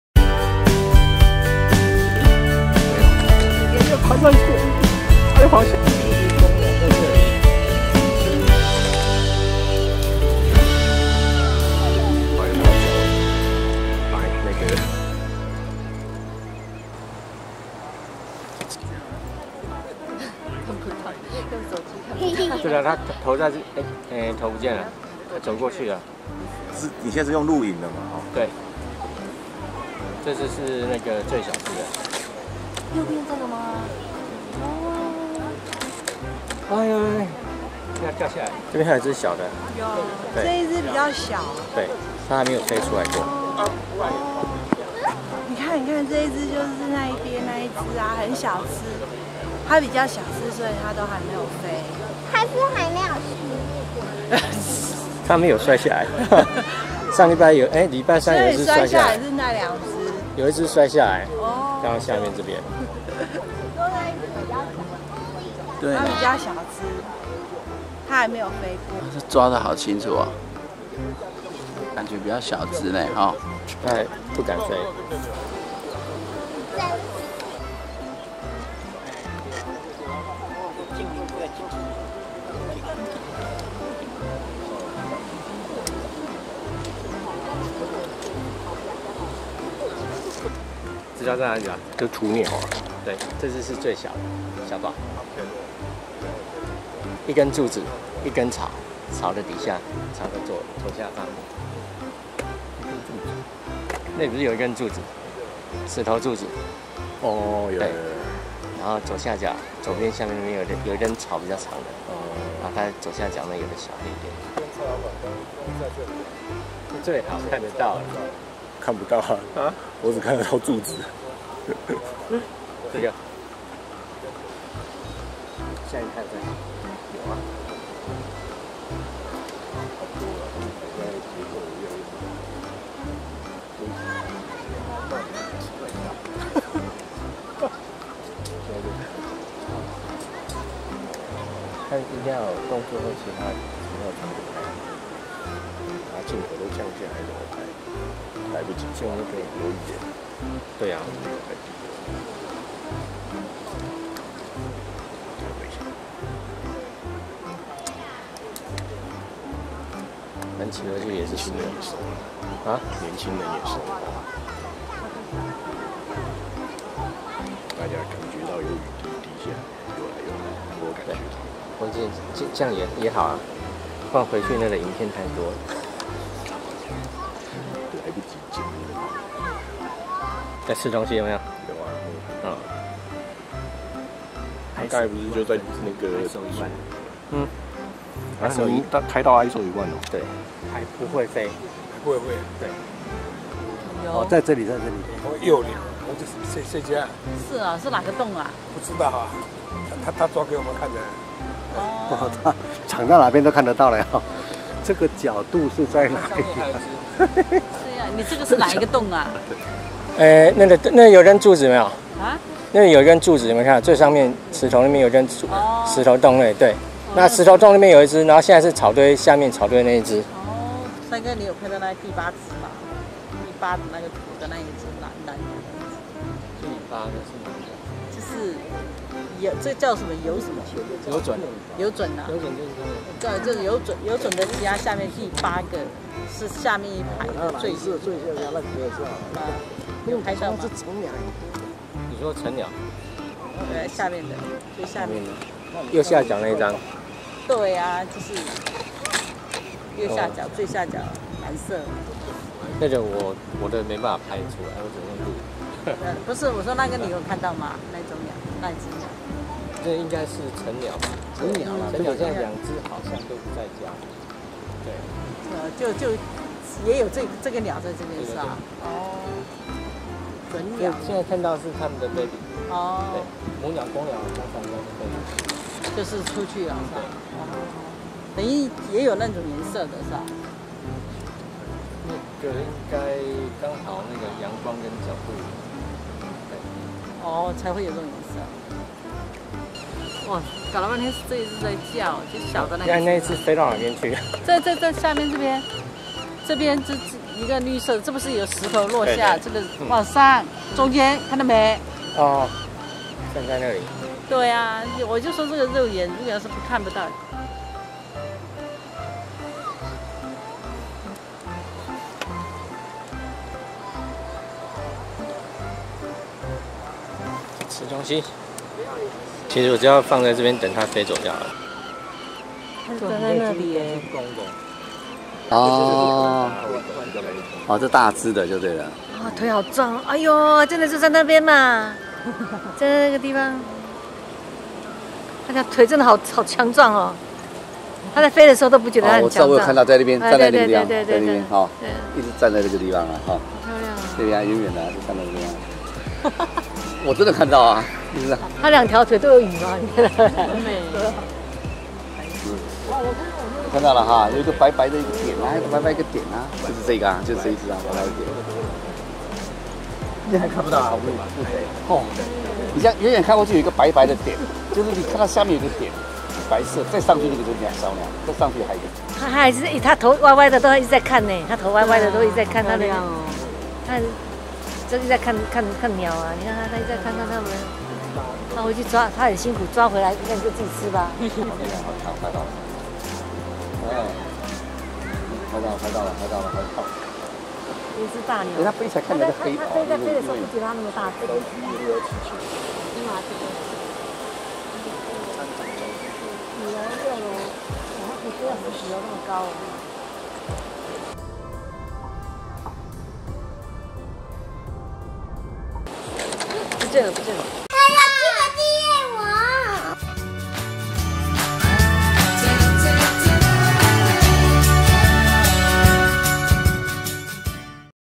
酋長岩遊隼_EP31_在家還不會飛的三寶_200510
2020年5月10日遊隼二寶秀,
大寶二寶已經很調皮的會在山頭上飛來飛去,
雖然飛行技術還不太好,
三寶還在巢穴附近不太能飛,
這一集應該是大寶欺負二寶的情形.